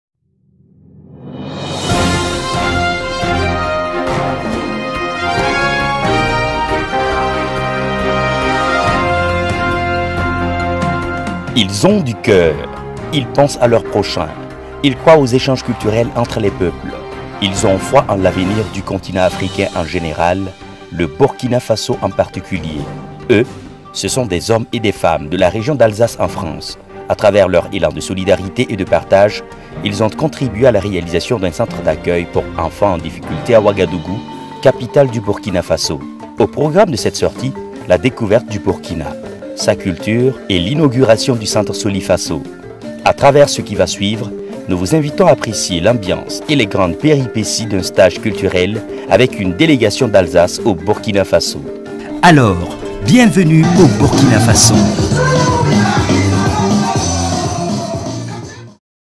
1. Stage Beoneema Europe-Afrique à Ouagadougou
Du 25 février au 10 mars 2012, l'association Beoneema Europe-Afrique a organisé un stage de danse et de percussions africaines au centre de formation Solifaso de Ouagadougou (Burkina-Faso)🇧🇫.

Ce reportage en plusieurs parties retrace ce beau voyage culturel et solidaire auquel a participé une vingtaine d'alsaciens avec Souleymane Zongo.

Réalisation : Elisée Soubeiga / Beni multimedia